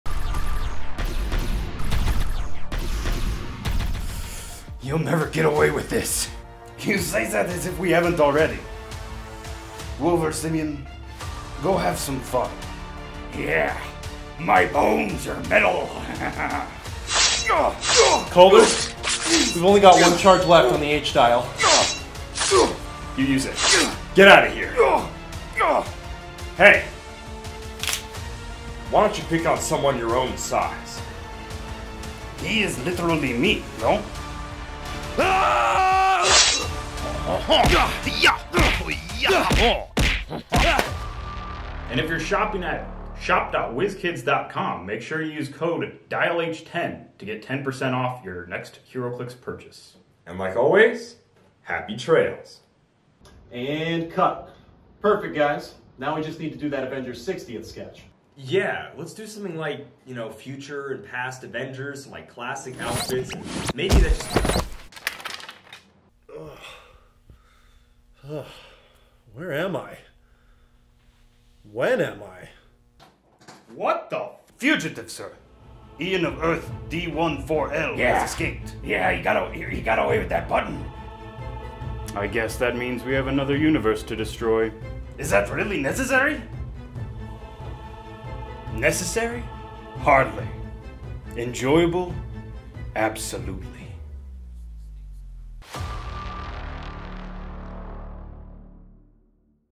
Heroclix A60th The Multiversal Masters of.. WHO!?
Dial H and our entire universe, including Heroclix, may be in grave danger. How will Kalder, Simeon, Ian and... Ian? What? Take down their greatest foes yet!?

Tune in tomorrow at 8 A.M CST for our Wizkids sponsored Avengers 60th unboxing!